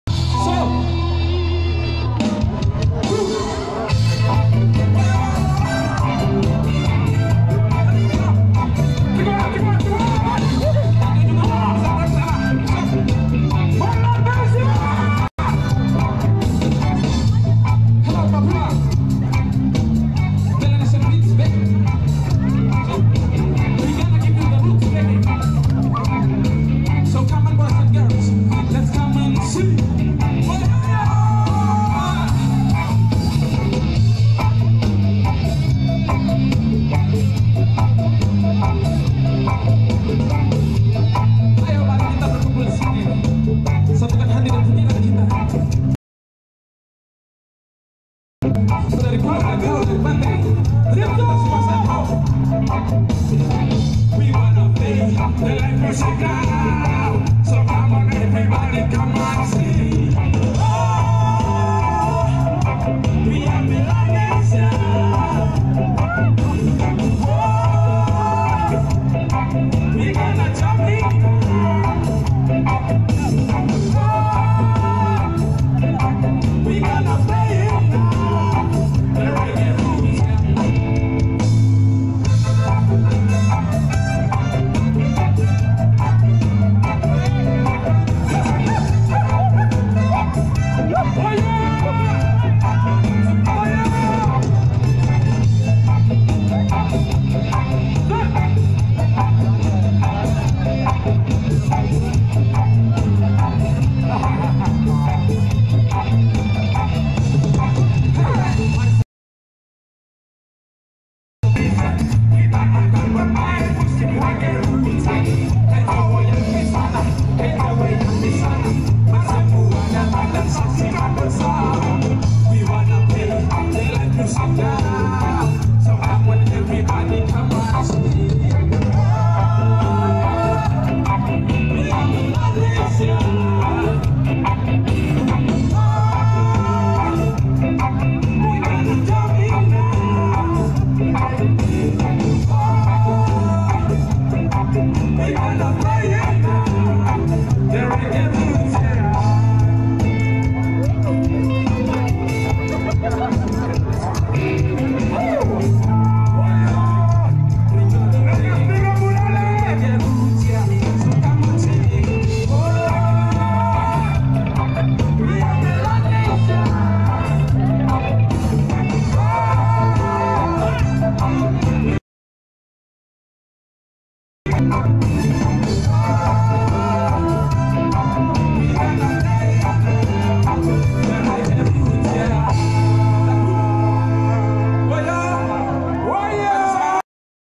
CONCER_LIVE MELANESIA_BEATS_BAND_
Roots Reggae di lapangan Trikora Abe
Papua_Indonesia
2018
Lagu Ciptaan.Melanesia Beats